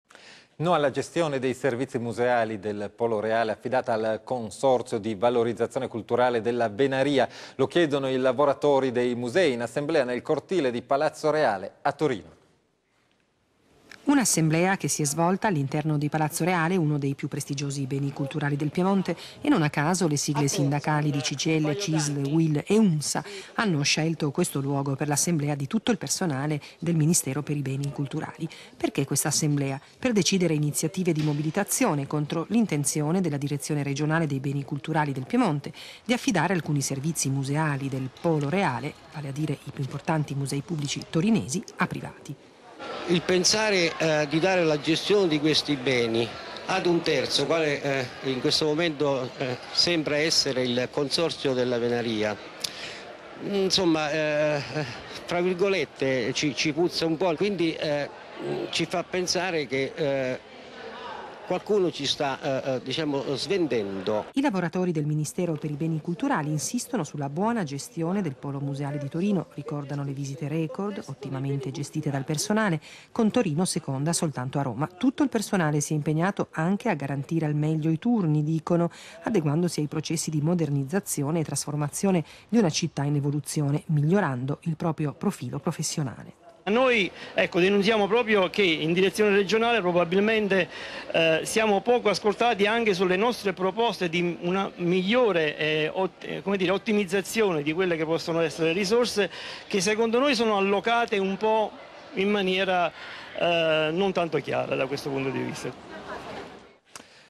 Assemblea lavoratori musei torinesi.mpg
La protesta di Cgil-Cisl-Uil Funzione Pubblica che temono un colpo di mano della Direzione Regionale dei Beni Culturali sul cosidetto "Polo Reale" dei musei cittadini.